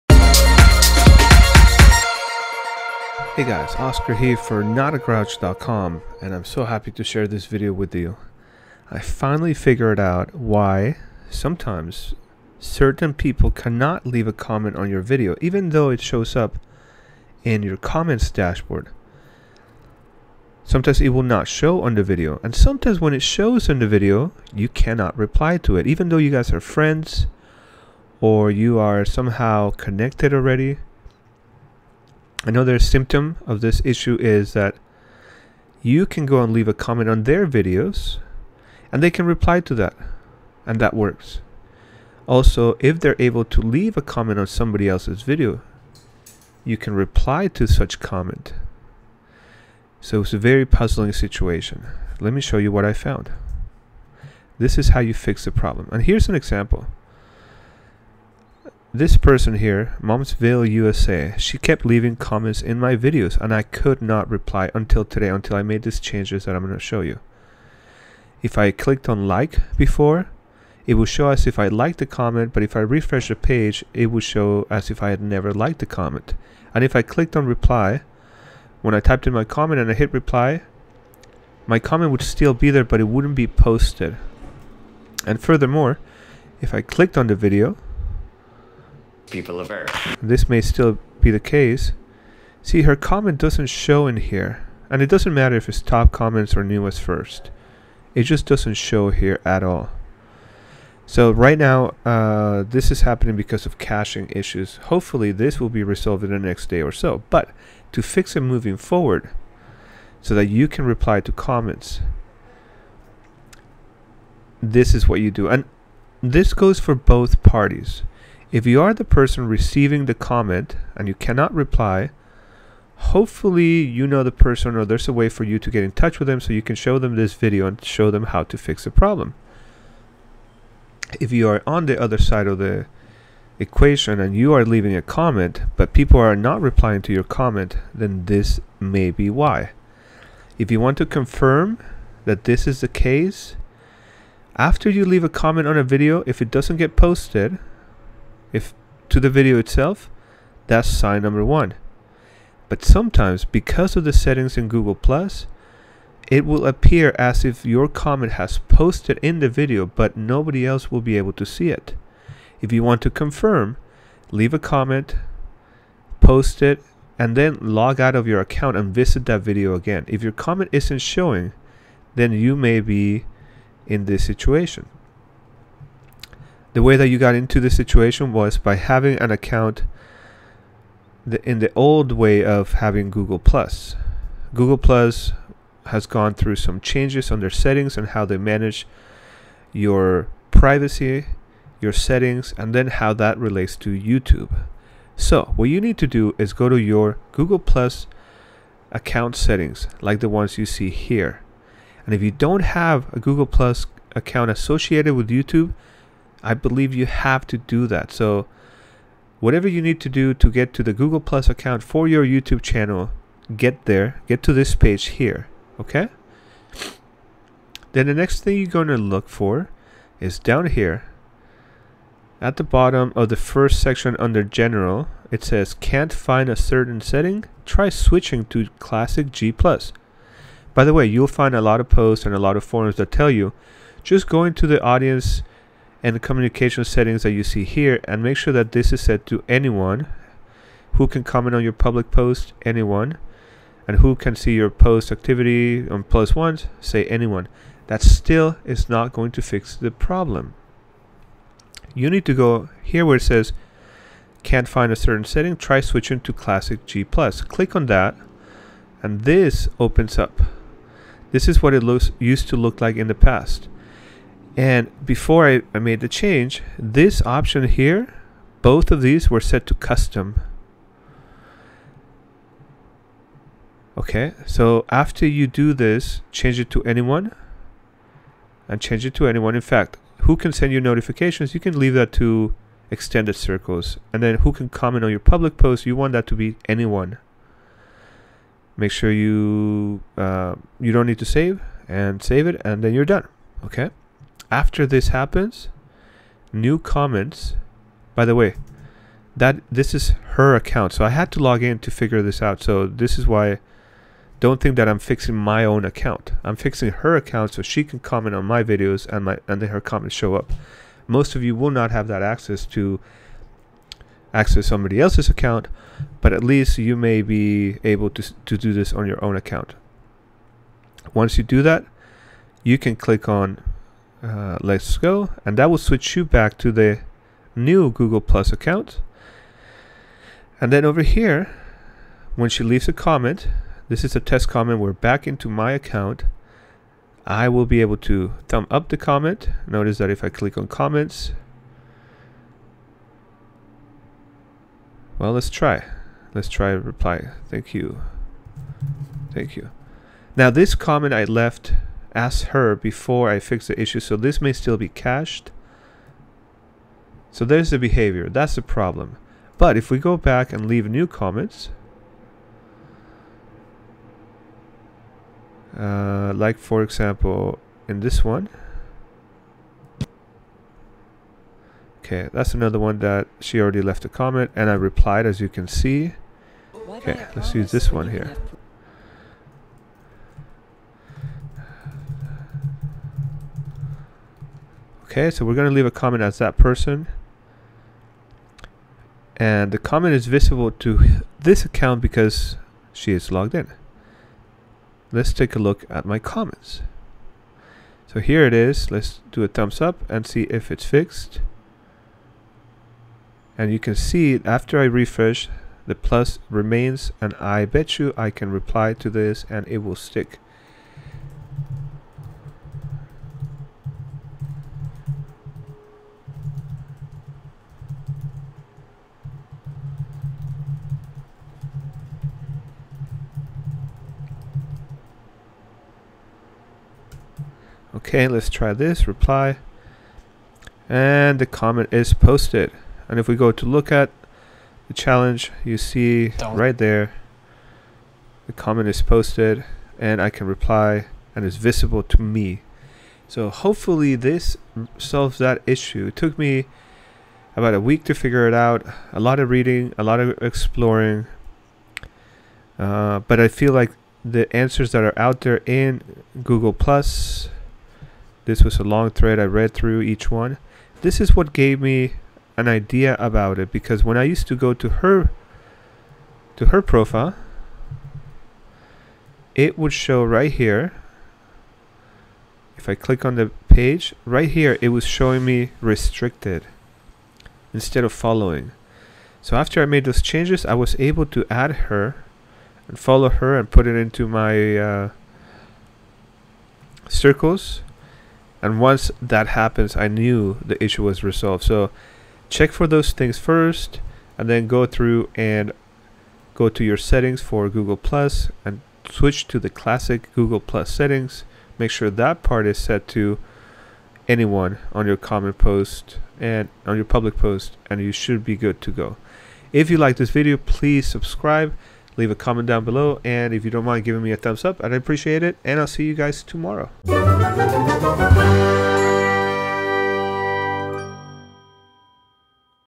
*Volume Alert* Fixed! Can't Comment or Reply. Can't See Comments on Youtube Videos
* New issue "Comment failed to post" -- see the info after this description below. Too long didn't read? Too long didn't watch? - get this app and 99% of your problems will be fixed -- or read on for more details.

By the way, if you found this useful, take a trip to my blog and see if you like anything else over there.

There is a very annoying situation that happens on YouTube where sometimes you can't reply to comments people make on your videos. Another symptom is that people leave a comment on your video but it is not visible to everyone else. After scouring the web for about a week and trying different things, I found the solution. Here it is.

I know that YouTube and Google + are going major changes and comments are changing, but for now this works and has worked for a handful of people that found they had this problem.

New issue - Comment failed to post.
It is happening to a lot of people, including me.
One way to fix this is to refresh the account, or go to another video and then come back, or logging out and logging back in.

But sometimes doing all this still doesn't help and you still get the problem. The only relieable way I found to be able to respond to comments on the first try is to use the alerts on the top right corner and this app - That app will add a "reply" link to the comment alerts on the notification area and you can reply right there. It has been working for about 2 weeks for me now consistently. -- Hope this helps you too.